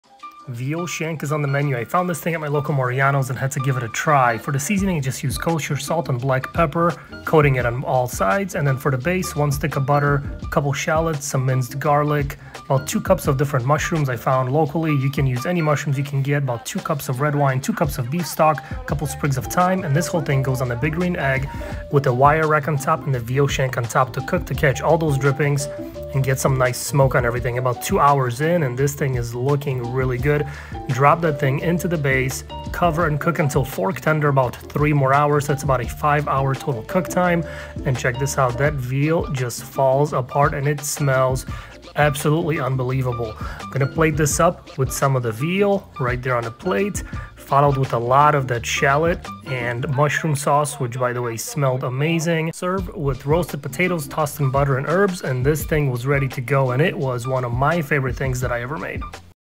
Veal Shank
This was easily one of my favorite things that I cooked on my page. Veal shank in a mushroom and shallot sauce.⠀shank
I found the shank at my local @marianosmarket and had to get it. I seasoned it heavily with kosher salt and pepper and set it aside. For the base, I added 1 stick of butter, 4 shallots cut in half, about 2 cups of mushrooms, minced garlic, about 2 cups of red wine and 2 cups of beef stock. I smoked the shank over the dutch oven for about 2 hours on the @biggreenegg .⠀
After it had a nice color, I dropped the shank in the dutch oven, covered, and cooked until fork tender, about 3 hours.⠀
And it turned out AMAZING! The meat was super tender and so flavorful. That mushroom and shallot sauce was delicious and went so well with the veal. Served with some roasted potatoes tossed in butter and herbs, and this made for an amazing dinner. Def making this again.